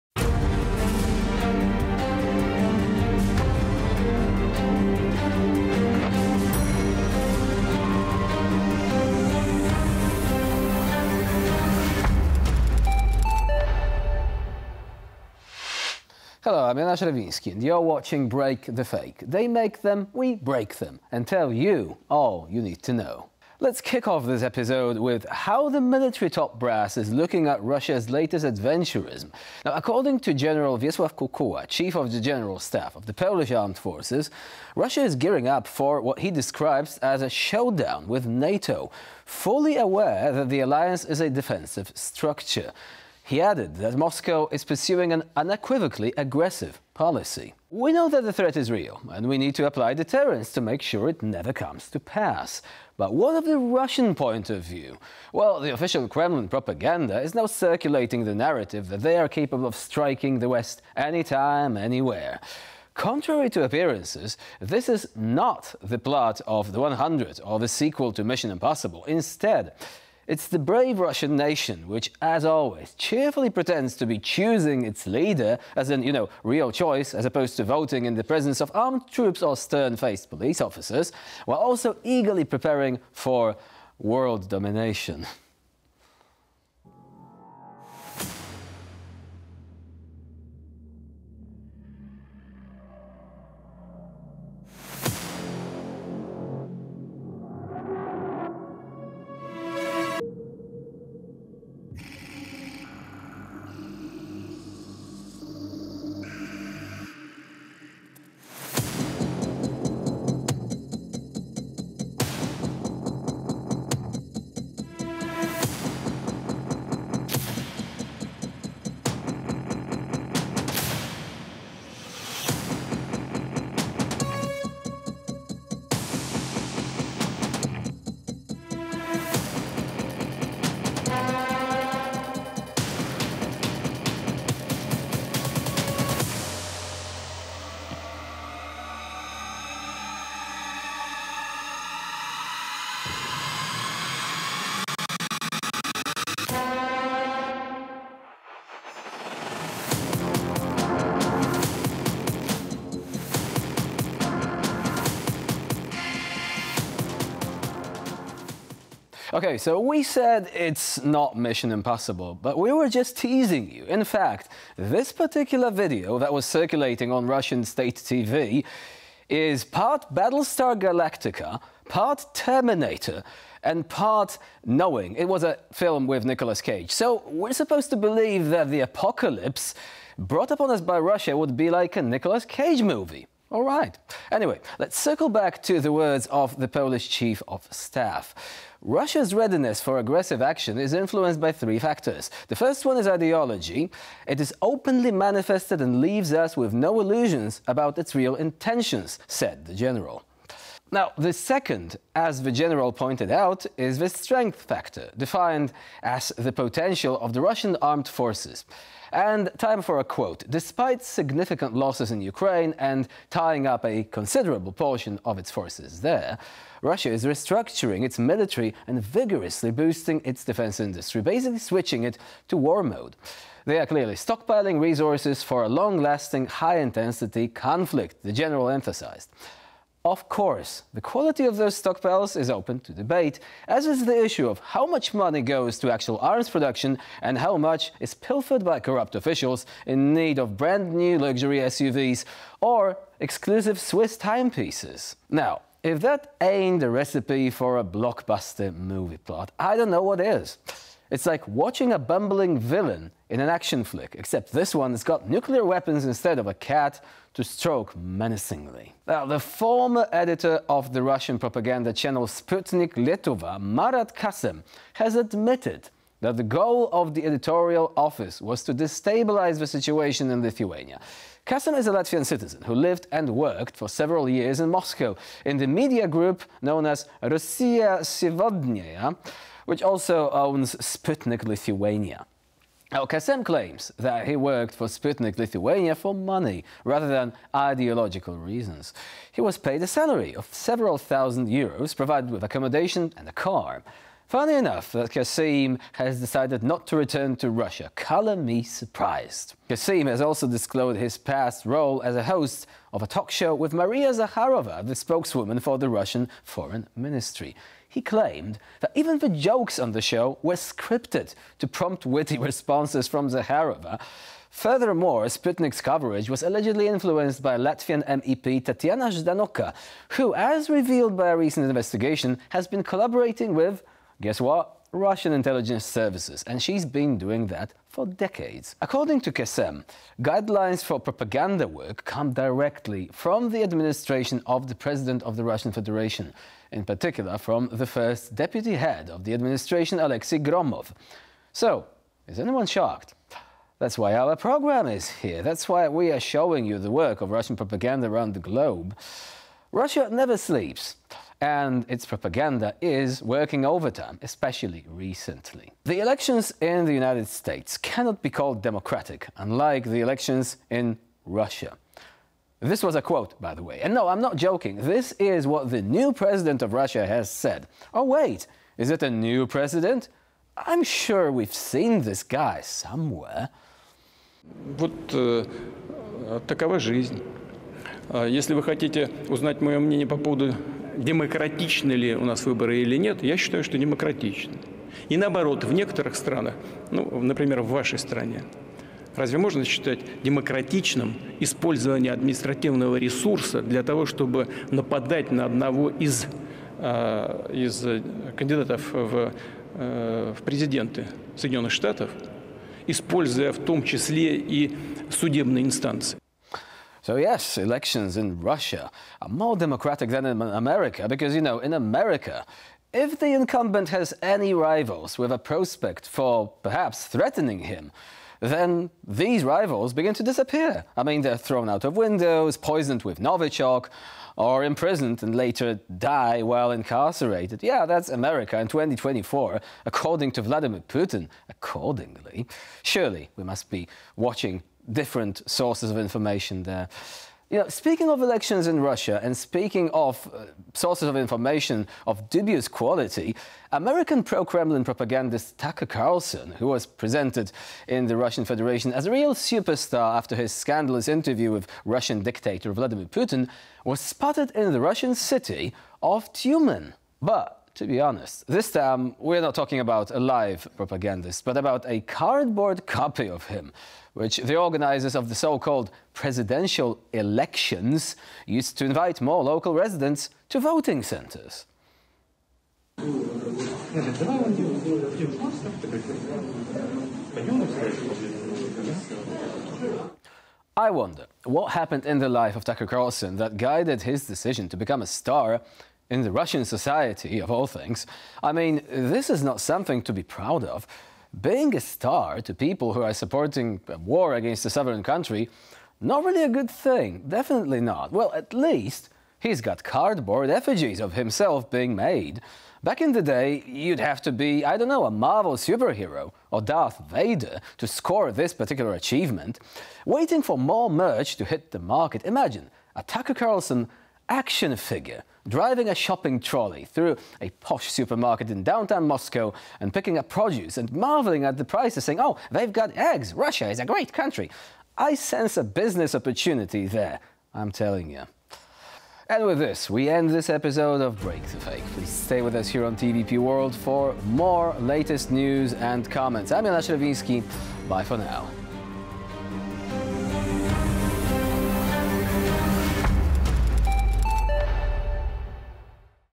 Ideology, Military Might, and Corruption in the Crosshairs | Break the Fake
Russia's readiness for aggressive action is influenced by three factors.